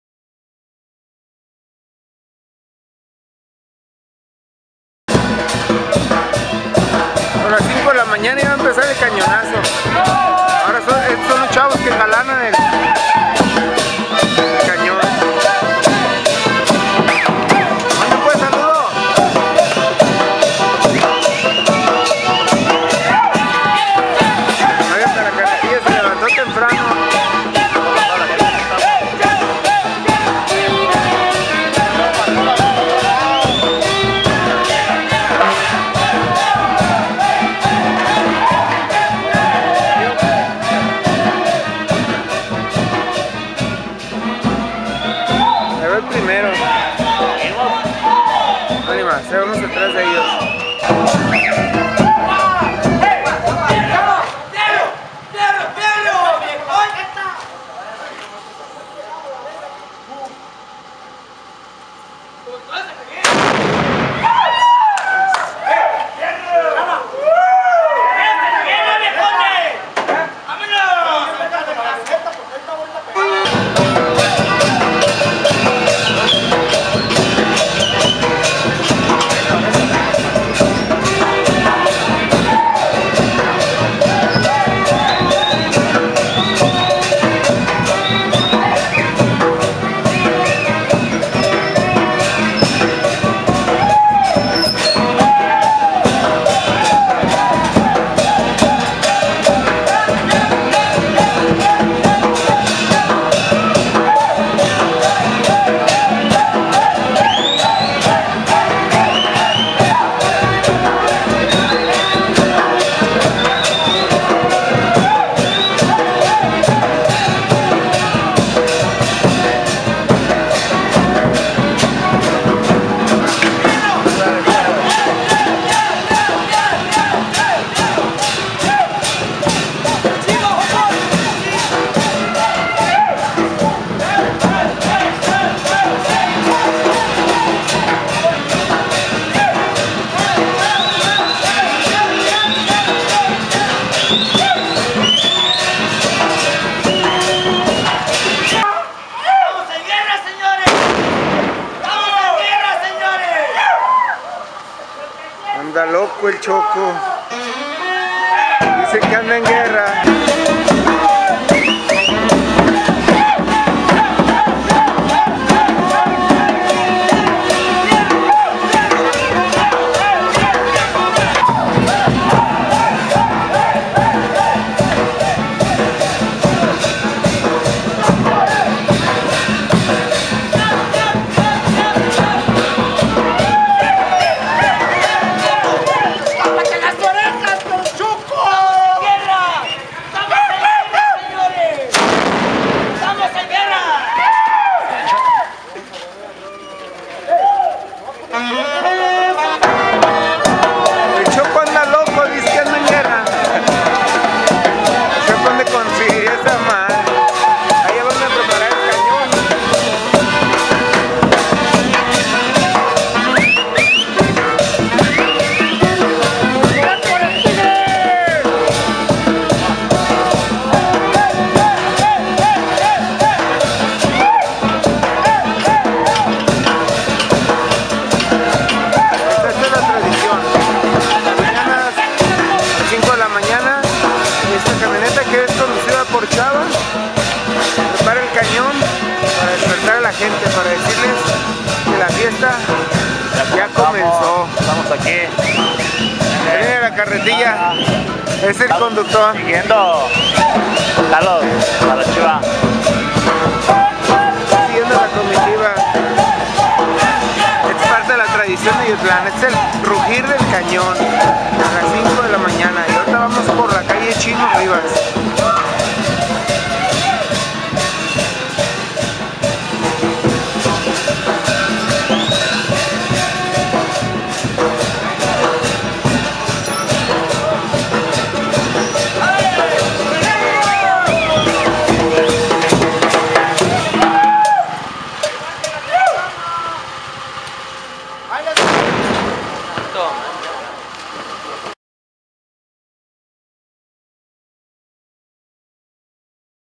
Primer Cañonazo dia de Campo 2015 Primer Paseo
Comenzó la fiesta en Ayotlán y este cañón avisa a los pobladores y visitantes de este pueblo que hoy comienza la cuenta regresiva para el "Día de Campo". Esta tradición data del hace cientos de años y que al paso de los tiempo a perdurado, si quieres saber mas sobre esta tradición visita